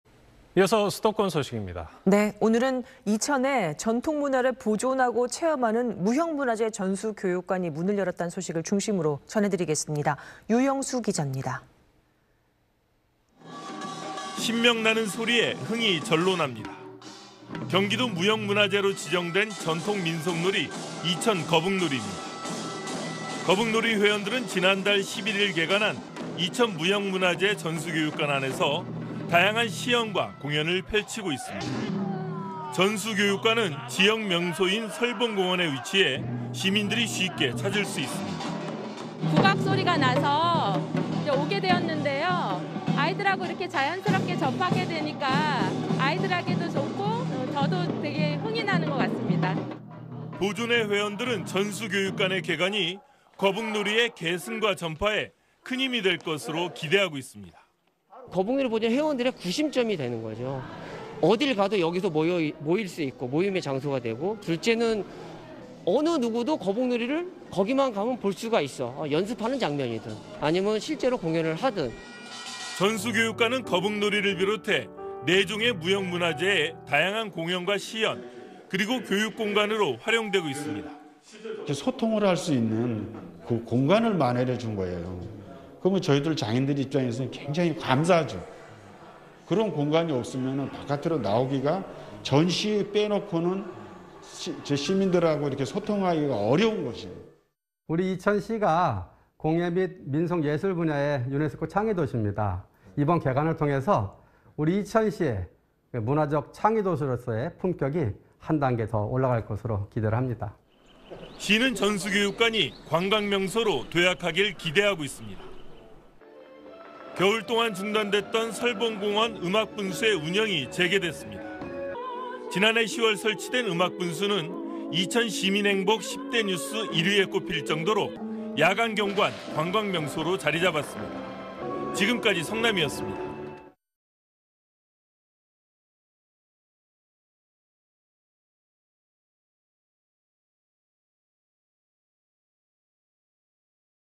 이천시, '무형문화재 전수 교육관' 개관…'설봉공원' 위치 / SBS
오늘(13일)은 이천에 전통문화를 보존하고 체험하는 무형문화재 전수 교육관이 문을 열었다는 소식을 중심으로 전해드리겠습니다.
유영수 기자입니다.